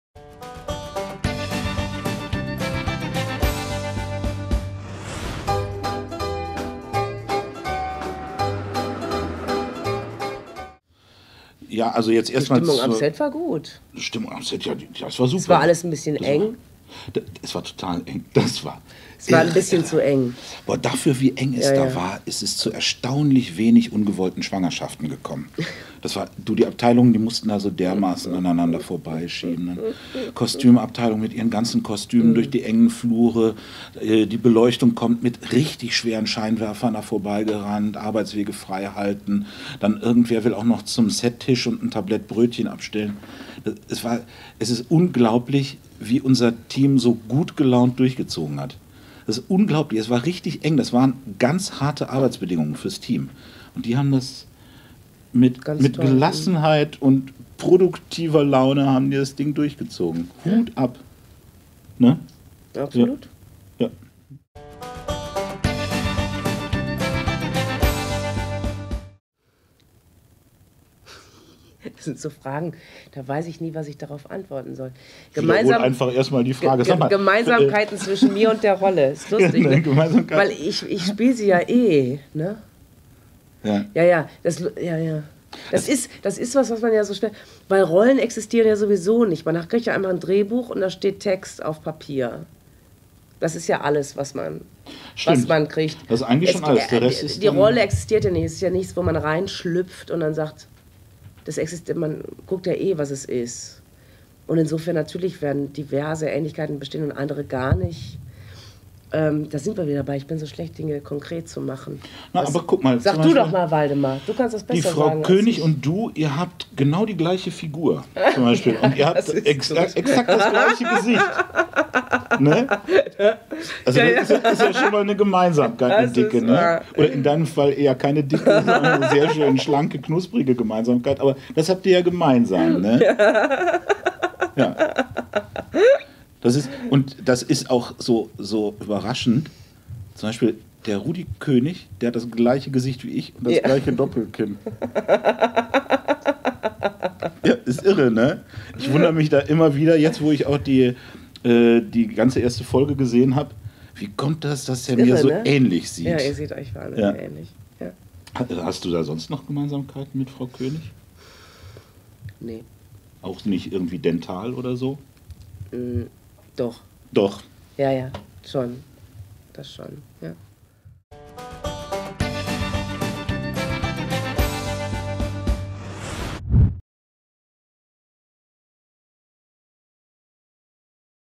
Die LottoKönige: Interview Sandra Borgmann und Waldemar Kobus Part2
FSK: 6

Die LottoKönige, die neue WDR Serie ab Sonntag, den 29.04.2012 jeden Sonntag um 21.45 Uhr im WDR. Die DVD und BluRay zur neuen Comedy Serien mit u.a. Oliver Wnuk, Sandra Borgmann und Waldemar Kobus ist ab dem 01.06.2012 im Handel und als Download erhältlich.